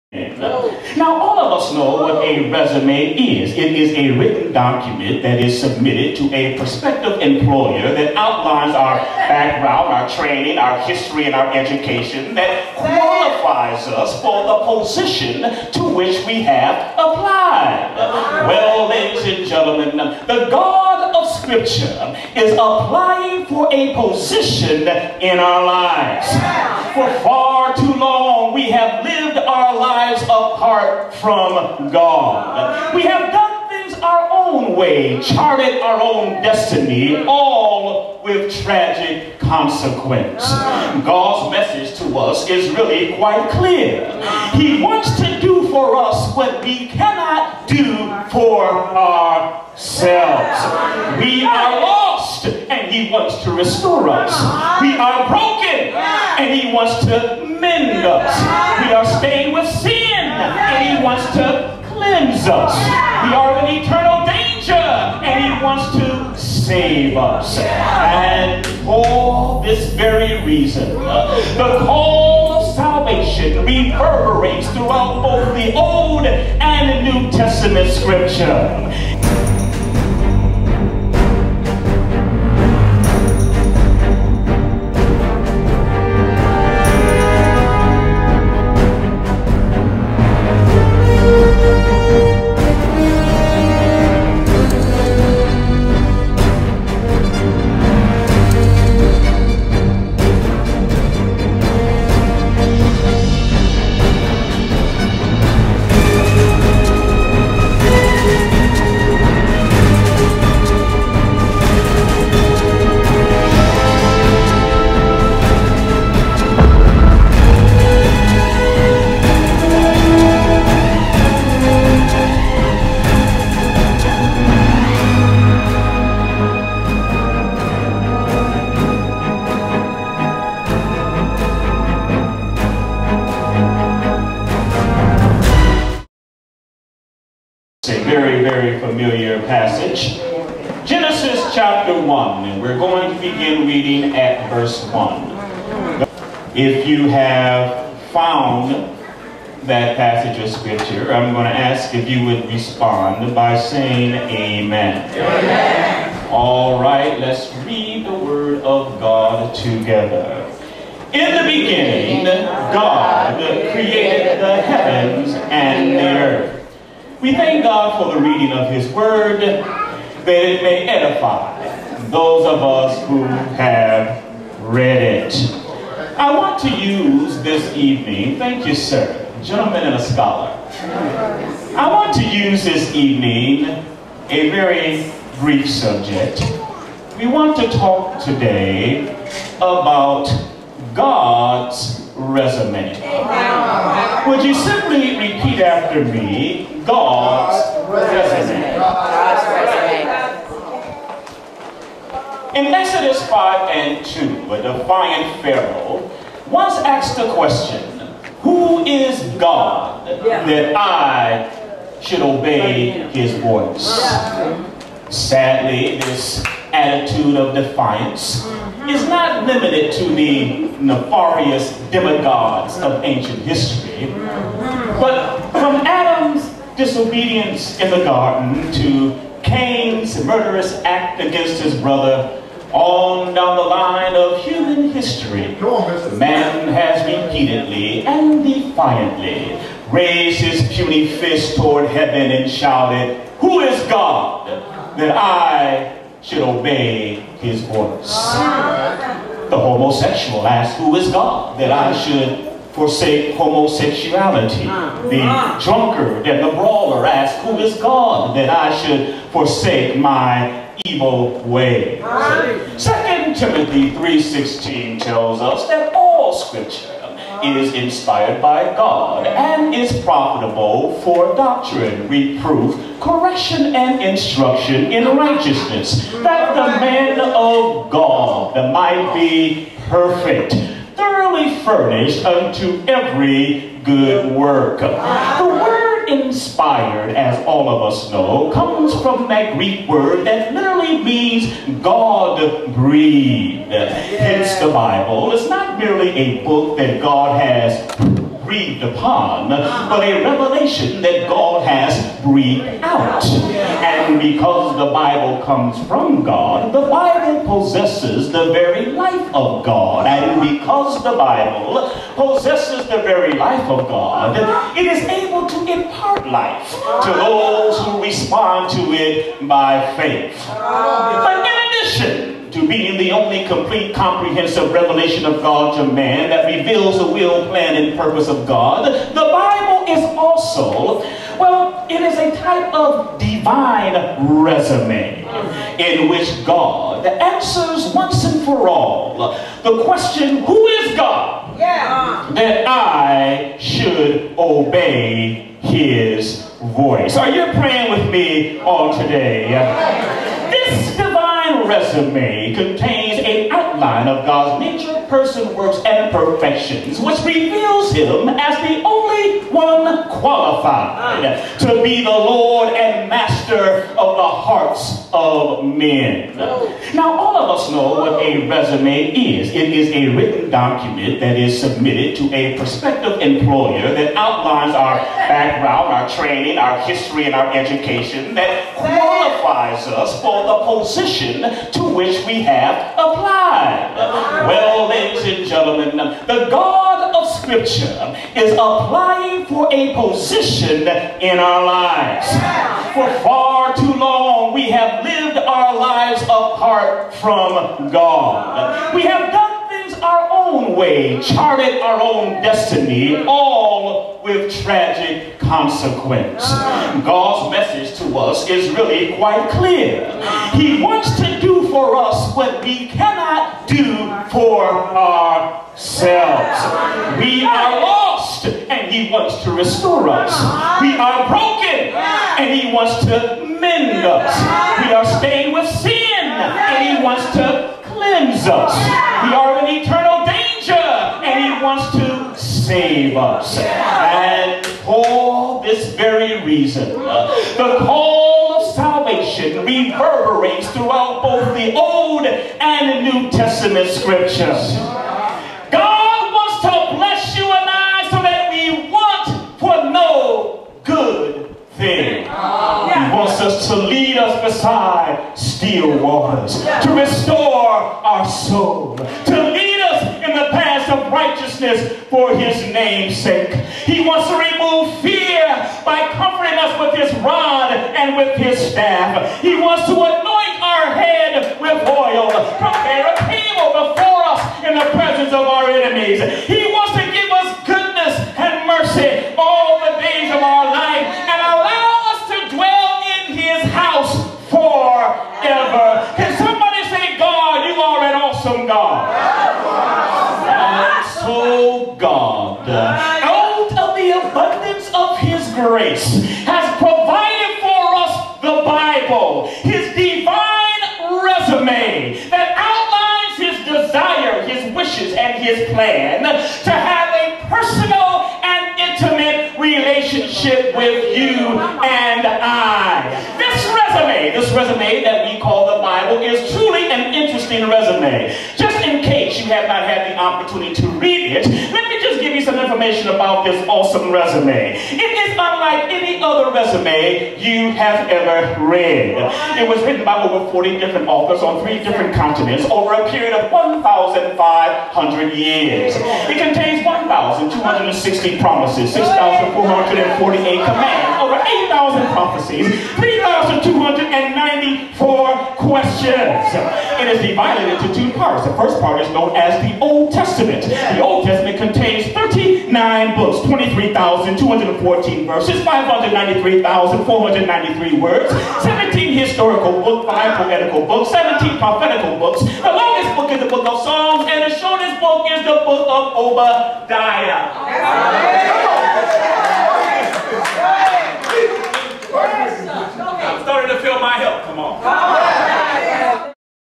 Sermon: God's Resume
In addition to being the only complete comprehensive revelation of God to man, the bible is also a type of divine resume that outlines God's nature, works and perfections that qualify Him to be Lord and master over our lives. In this sermon entitled "God's Resume", Bishop Ricky Holmes brings out some fascinating truths about the word of God and the God of the word.